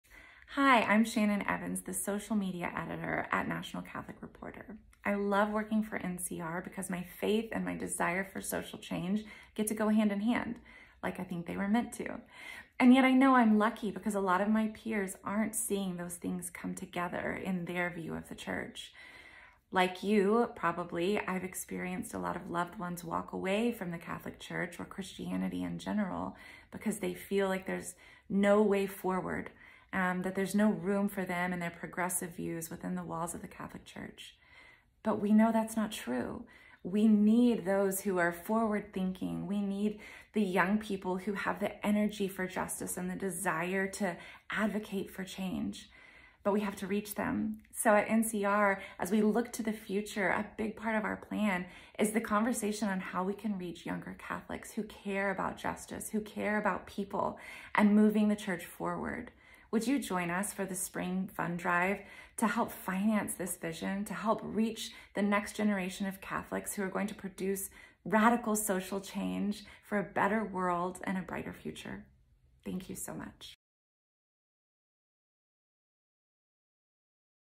From Shannon Evans, NCR's Social Media Editor 2022
NCR Social Media Editor Shannon Evans talks about reaching a new generation of readers.